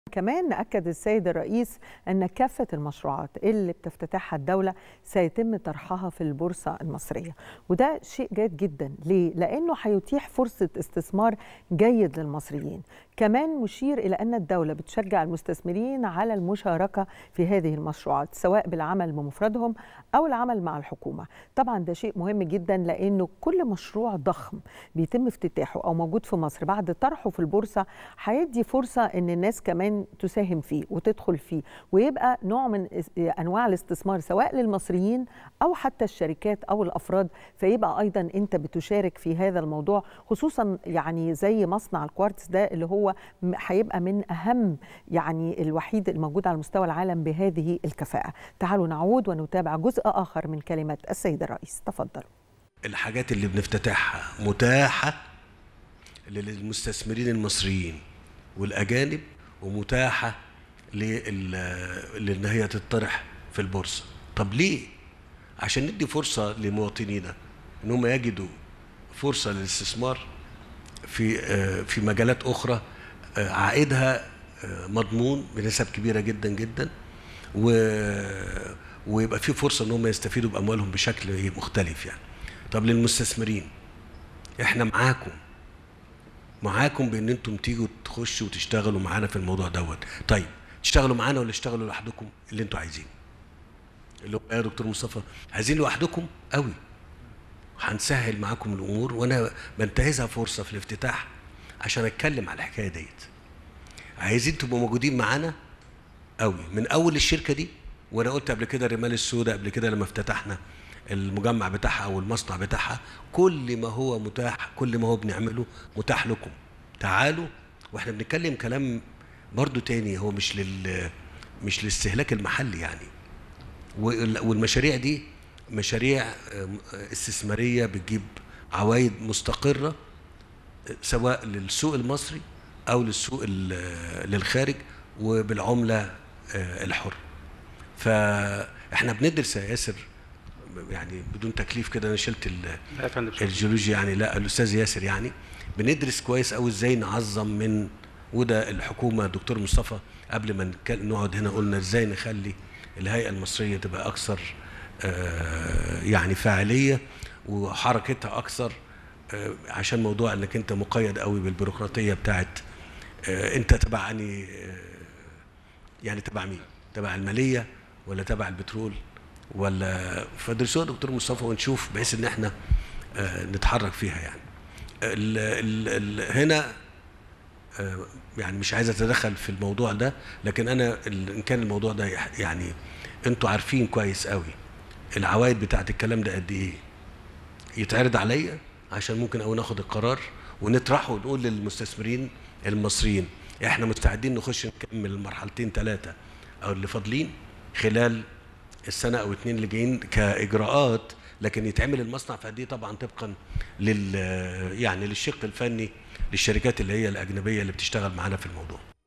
الرئيس السيسي: طرح كافة المشروعات التي تفتتحها الدولة في البورصة لإتاحة فرصة استثمار جيدة للمصريين
التاسعة | الرئيس السيسي: طرح كافة المشروعات التي تفتتحها الدولة في البورصة المصرية لإتاحة فرصة استثمار جيدة للمصريين